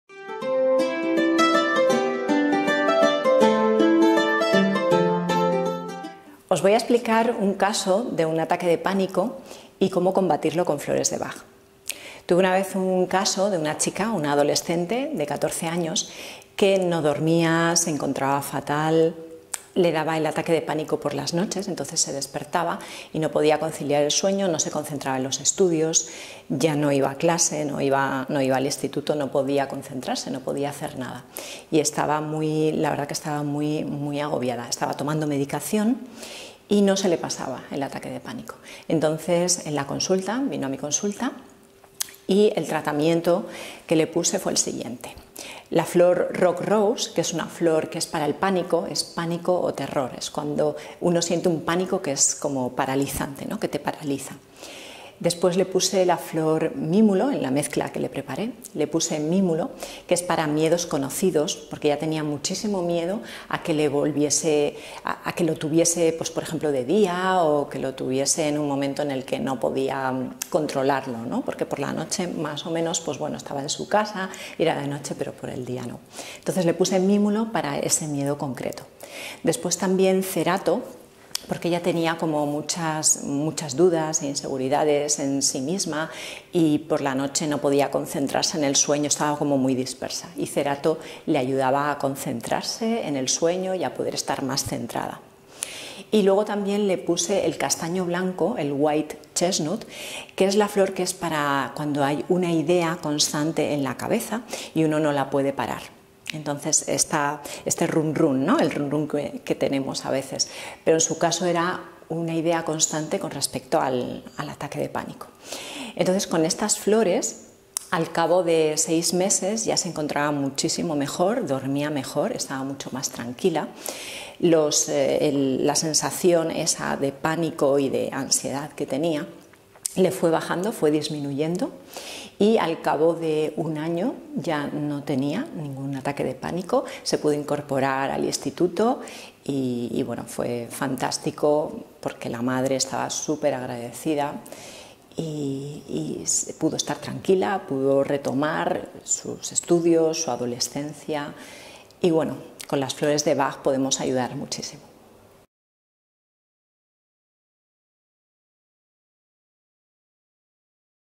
Ataque de pánico y Flores de Bach
Susana Sardón, terapeuta floral y nuestra formadora en el curso de Flores de Bach, nos pone otro interesante ejemplo de cómo las flores de Bach pueden ser muy efectivas en todo tipo de casos. Esta vez, frente a los ataques de pánico.

Si estás interesad@ en este curso, puedes acceder a toda la información a través del link que te dejamos a continuación: